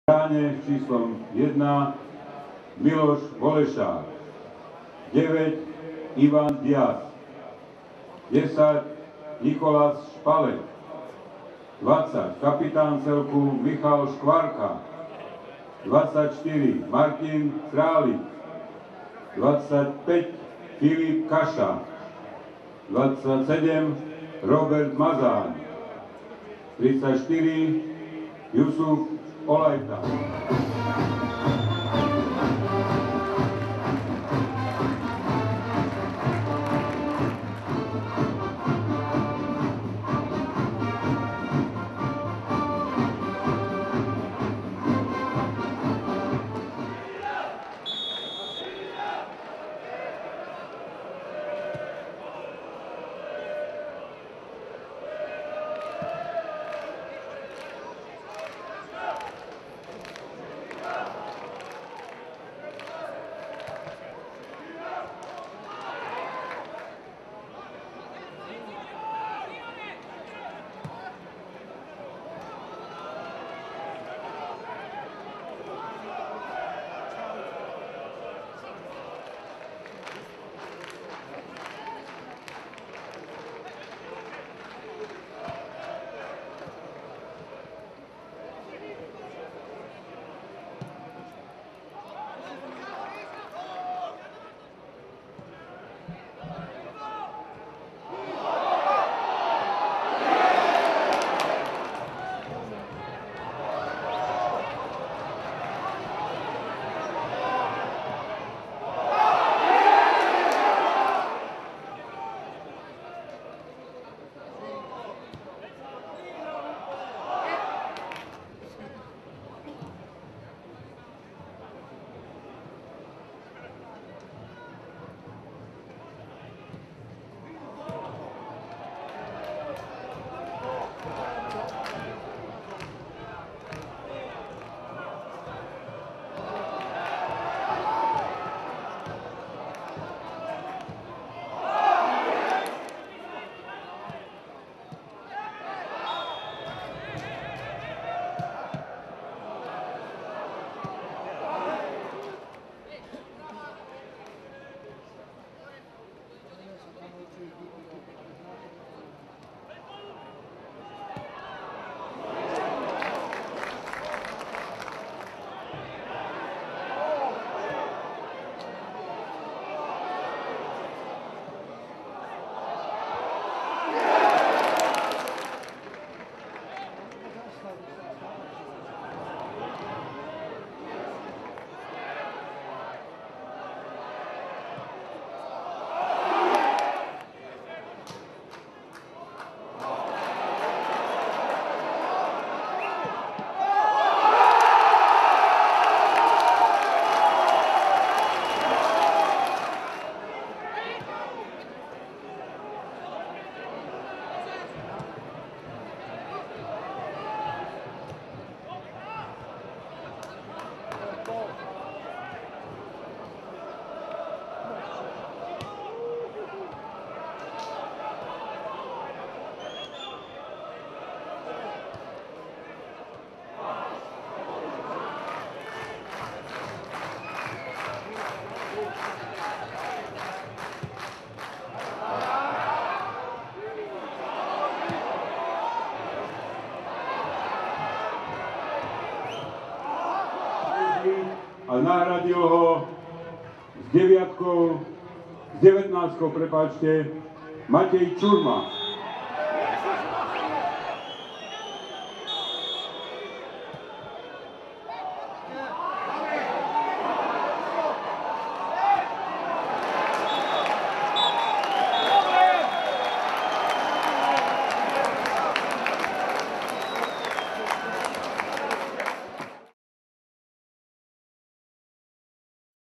MFK Ružomberok - MŠK Žilina 1:0 (0:0)
Gól: 84. vlastný, ŽK: 86. Kochan (Ružomberok), rozhodoval Kráľovič, 1 530 divákov
Zostava MŠK: Volešák – Chvátal (87. Jánošík), Lačný (63. Mráz), Špalek